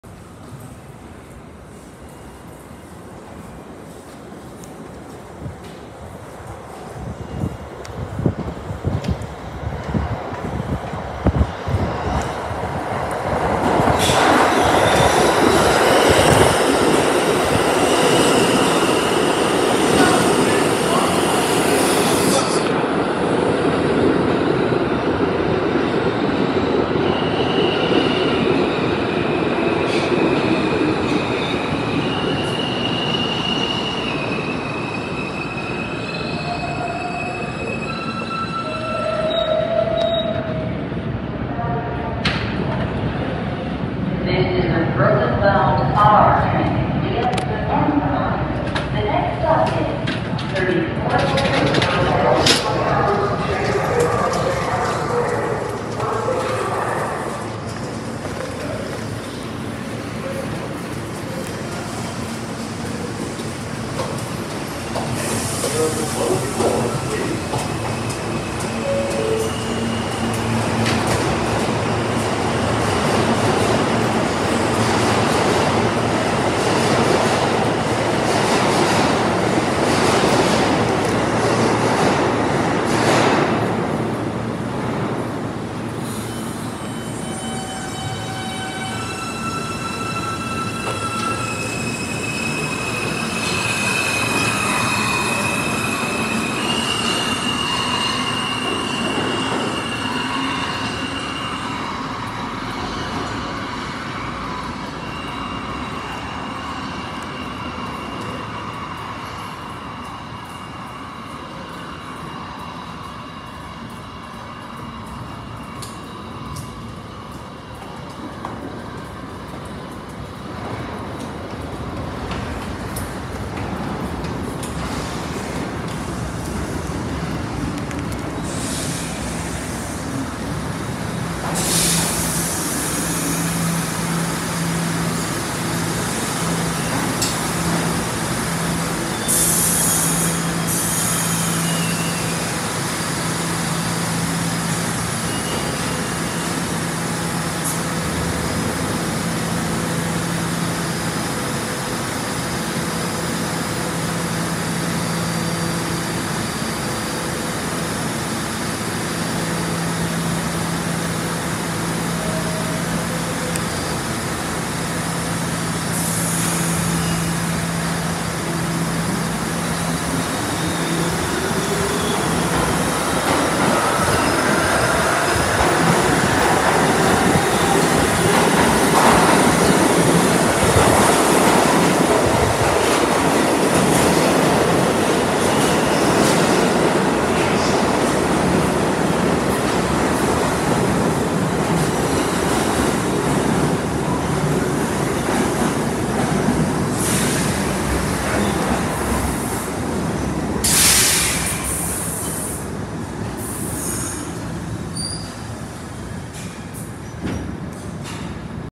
R160A-2 Half Cuomo (R) train via the (M) line/R68 (D) trains at 42nd street-Bryant park!
Shown here in this video is a Brooklyn bound R160A-2 Half Cuomo (R) train via the (M) line heading to Bay Ridge-95th street also shown 2 R68 (D) trains 1st one heading to Norwood-205th street 2nd one shown arriving heading to Coney Island.

Due to a R46 (W) train with brakes in emergency at 5th Avenue 59th street cause of mechanical issues whole Broadway line was a mess no (N)s and (W)s between Manhattan and Queens some ended at Times Square-42nd street other 96th street 2nd Avenue and (Q)s were sent local in Manhattan because this incident just happened when this (R) train reached Queens plaza it had no choice but to operate via the (M) line from Queens plaza to 42nd street Bryant Park then via the (B) line to Dekalb Avenue.

This video was taken Friday May 12th 2023 at 7:30am